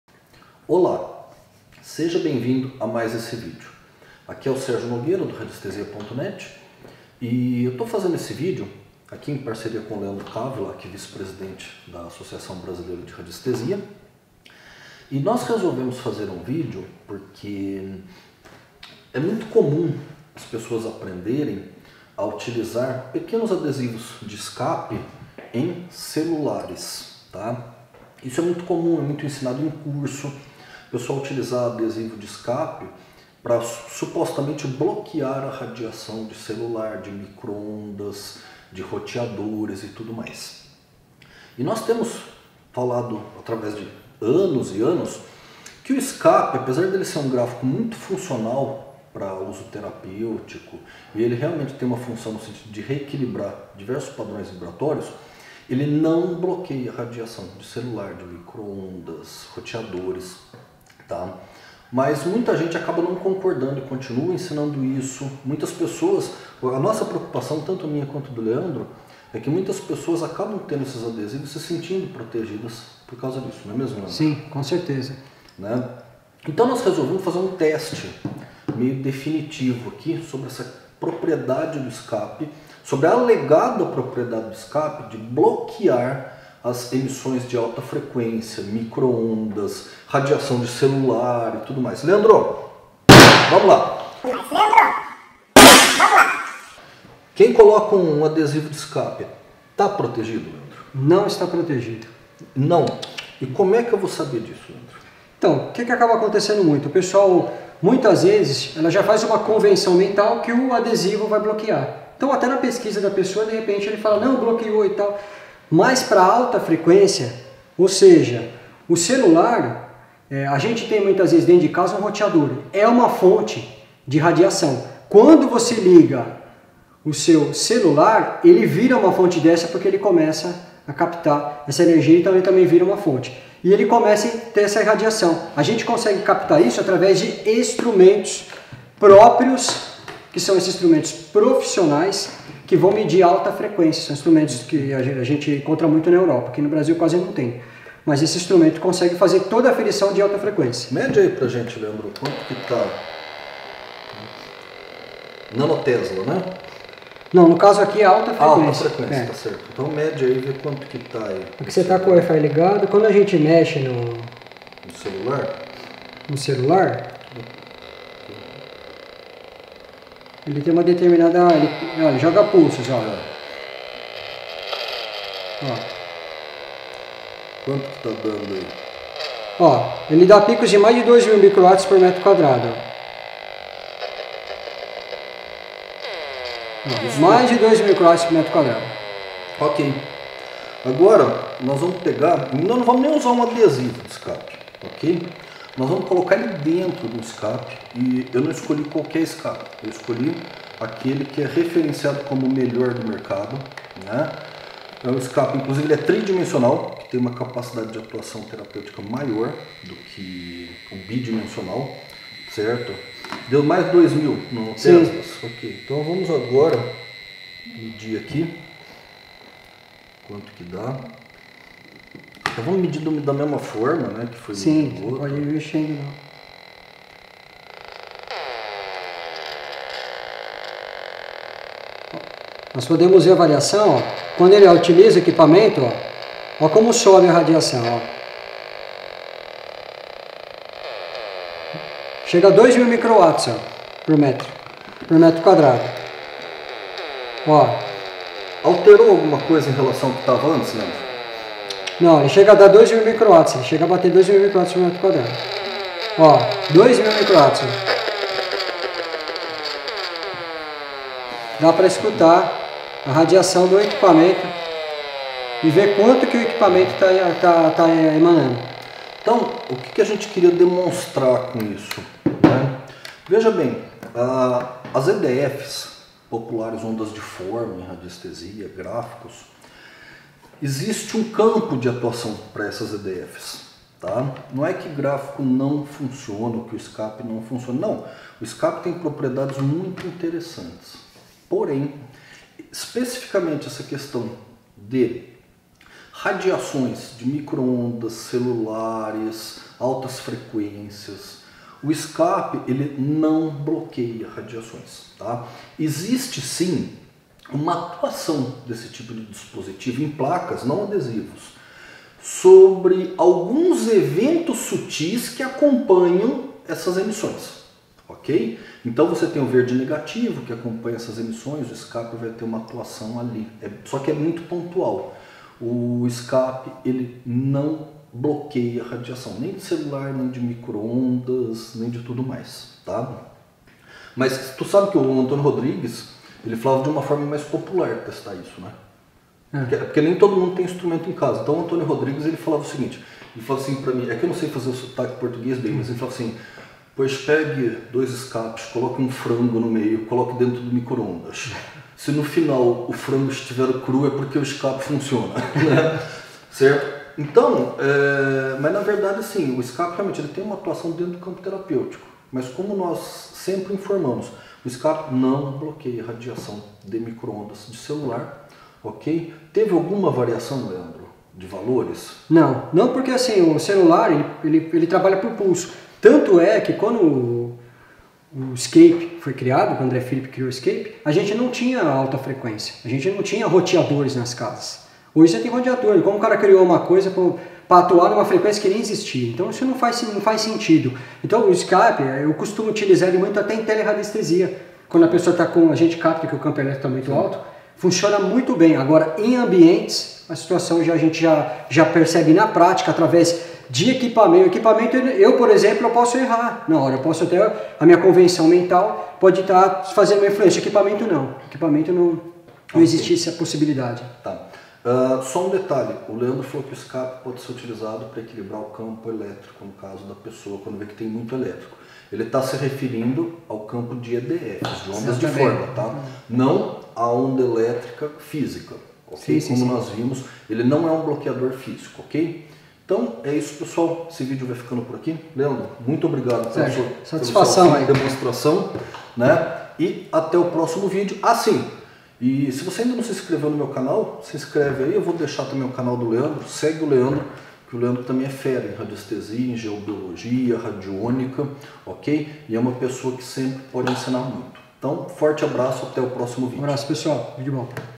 Gráfico de radiestesia em celular funciona na prática? Veja um teste definitivo.
Sim, muitas pessoas falam em usar adesivos de gráficos, especialmente o fantástico SCAP, em celulares, para bloquear as radiações dos aparelhos.
Mas será que funciona mesmo? O que podemos esperar?
Fizemos um teste para conferir a alegada propriedade do SCAP de proteger a pessoa de radiações de alta frequência.